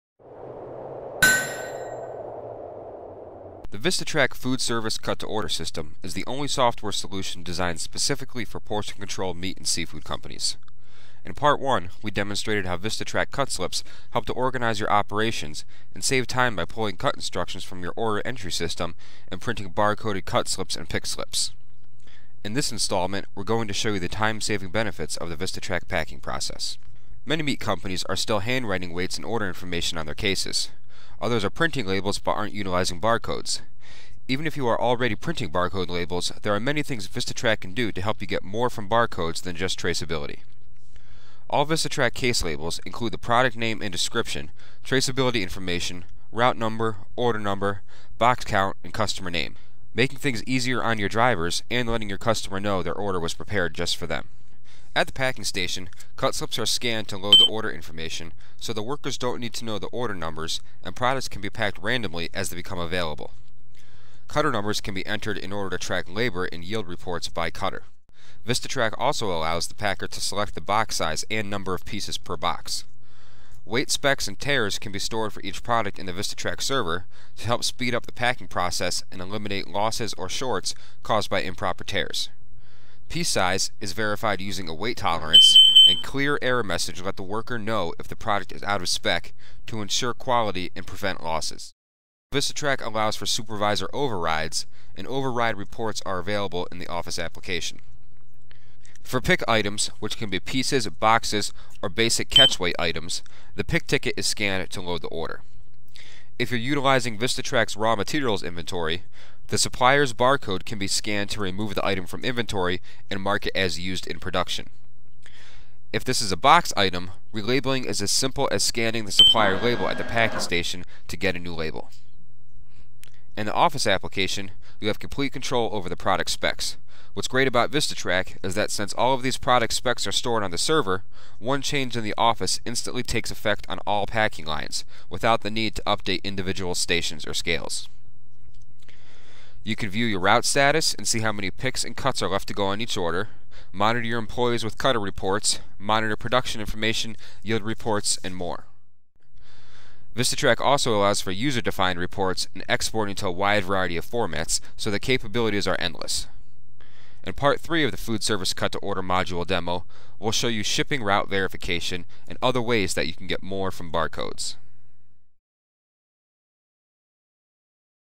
VistaTrac Cut To Order Part 2: Packing
This tutorial demonstrates the packing process within the VistaTrac Cut To Order module. It shows how the system uses barcodes to simplify and track the packing of individual products into a final case, ensuring a complete and accurate audit trail.

The video highlights the system's ability to efficiently handle packing in a "cut to order" environment. The user scans a barcode on a product and the system automatically links it to the order and records the item in the correct case. This process is key for accurate product traceability and maintaining an accurate raw materials inventory. The video emphasizes how the software is designed for the plant worker, with a simple, user-friendly interface that reduces manual data entry and human error.

This process is a core part of VistaTrac's comprehensive food traceability software, which provides "cradle-to-grave" supply chain traceability. The video also hints at the system's ability to create and print labels with 2D barcodes and integrate with business software like Quickbooks and NetSuite for a seamless flow of data from the plant floor to the front office.